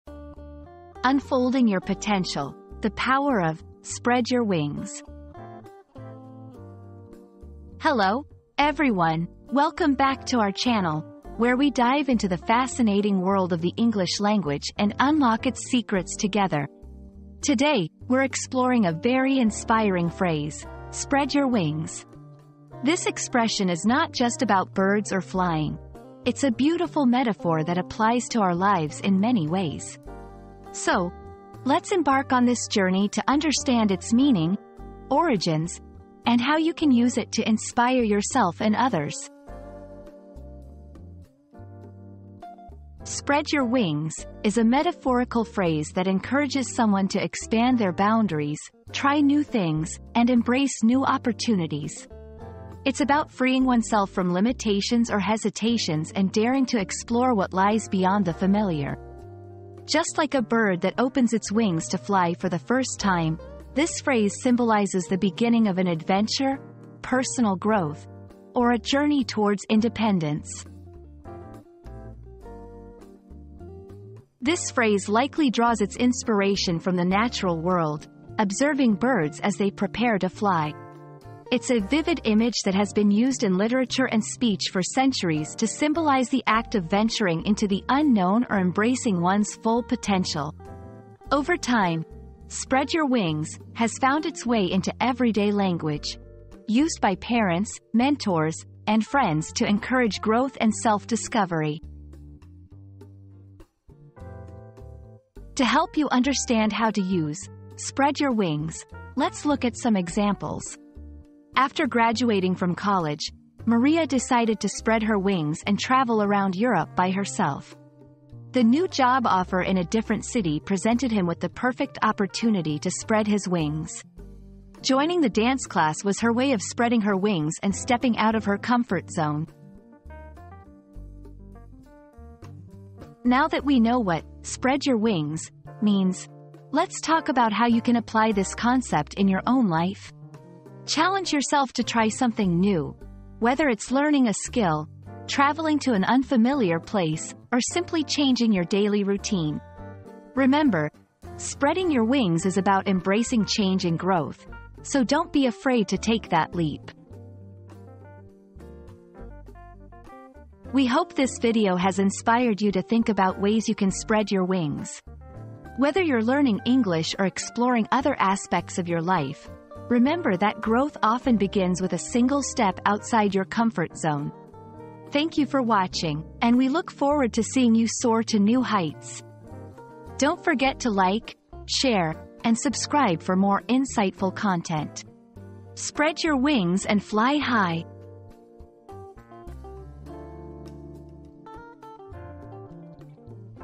Unfolding Your Potential: The Power of "Spread Your Wings"
Unleash Your Potential: Spread Your Wings • Discover the transformative power of spreading your wings and unlocking your full potential in this inspiring video.

00:00 • Introduction - Unfolding Your Potential: The Power of "Spread Your Wings"
00:42 • The Meaning of "Spread Your Wings"
01:13 • Origins and Usage
01:45 • Examples in Sentences
02:12 • Applying the Phrase in Your Life